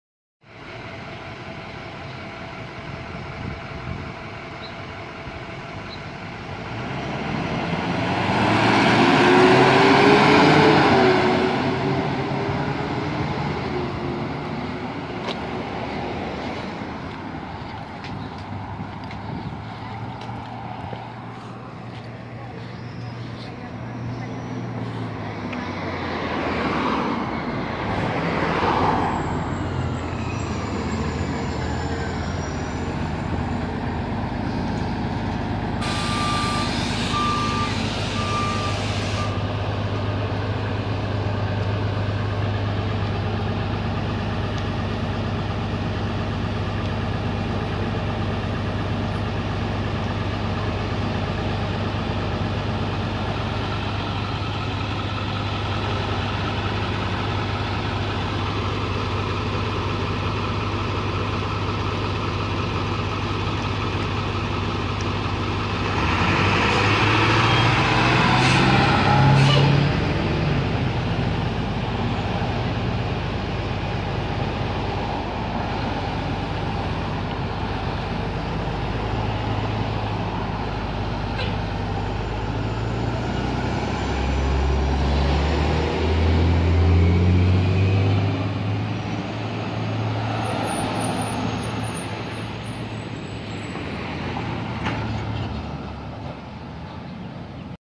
3 Buses on morning schedule
1st Bus - Access-A-Ride Bus
2nd Bus - MTA Orion VII Bus Q41
3rd Bus - Wheel Chair Mini School Bus Q056